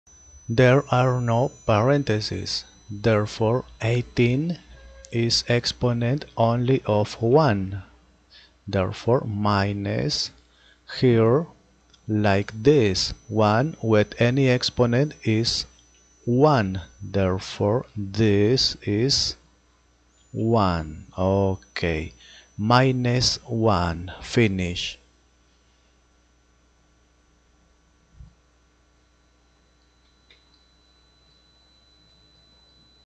minus 1 exponent 18 , -1 power 18 , negative number with positive exponent
negative number raised to a positive exponent without parentheses
whole number raised to a natural exponent without parentheses
negative base power m316